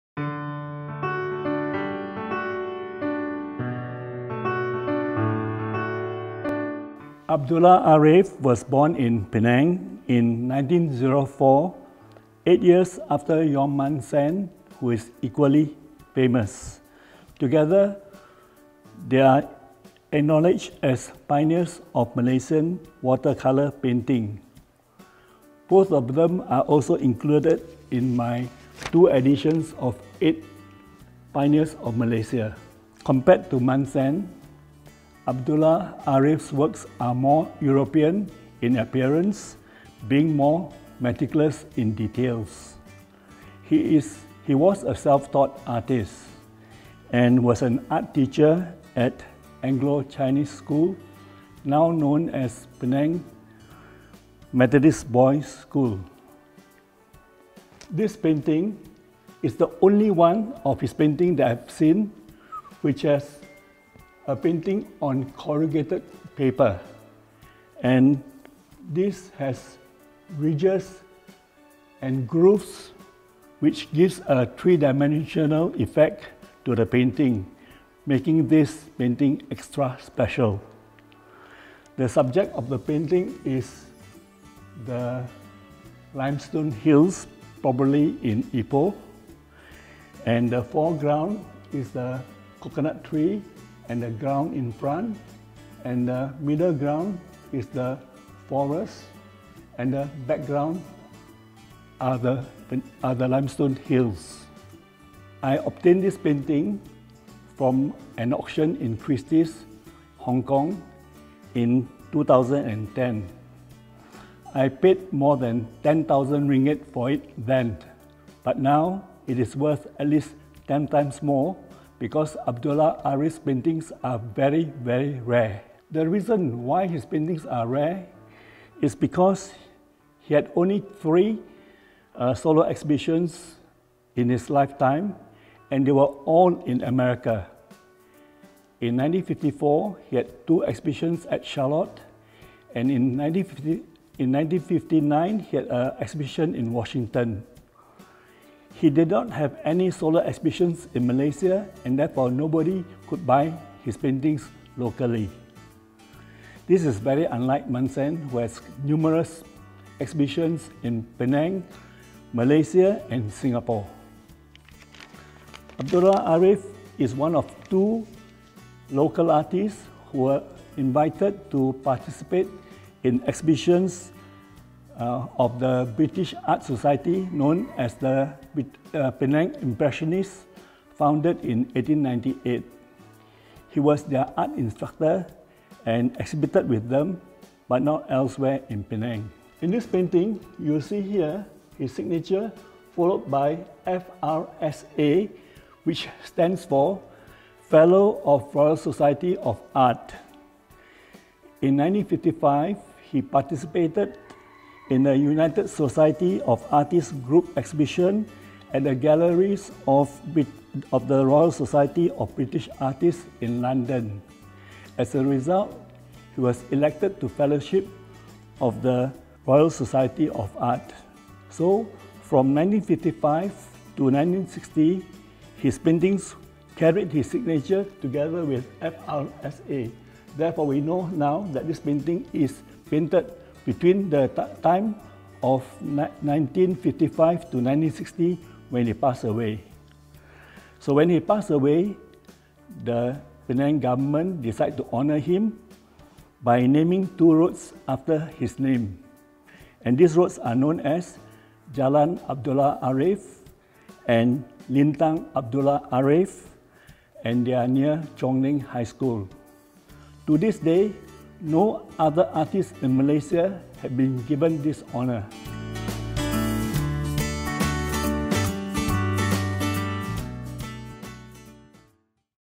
Permanent Collection of Penang State Art Gallery, ep. 1 - Presented by Dato' Dr. Tan Chee Khuan
Have you ever wondered what the permanent collection of our Penang State Art Gallery (PSAG) looks like?

In Episode 3 of Highlights of the Permanent Collection of PSAG, esteemed patron of the arts Dato' Dr. Tan Chee Khuan presents to us a brief glimpse of some of these works of art in the collection and the inspiration behind these treasures, not to mention anecdotes and insights.

This series is curated by Ivan Gabriel and Open Studios Penang.